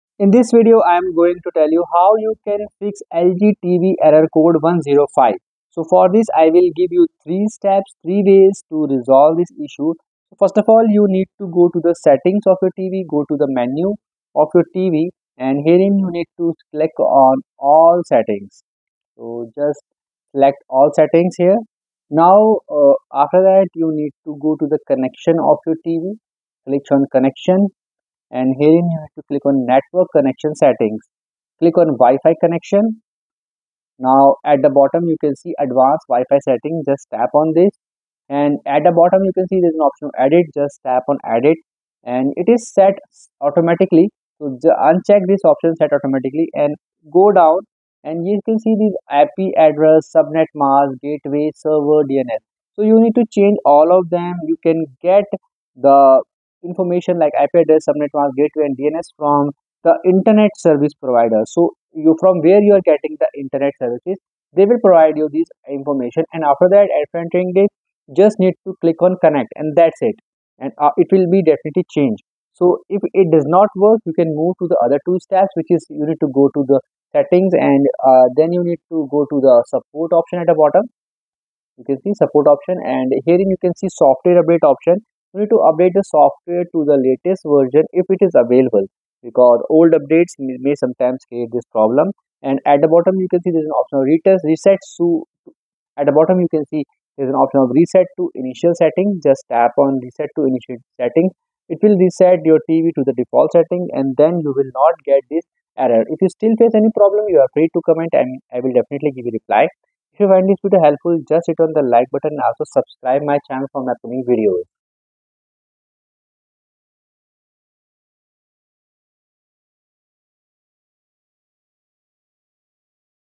3 Ways To Fix LG TV Error Code 105
Step – 1:- Change DNS Settings

• All Settings
• Connection
• Network connection settings
• Wi-Fi Connection
• Advanced Wi-Fi Settings
• Edit
• Uncheck “Set Automatically”
• Enter IP Address, Subnet Mask, Gateway and DNS Server as per your internet service provider and click on Connect

Step – 2:- Software Update

Step – 3:- Reset Factory Settings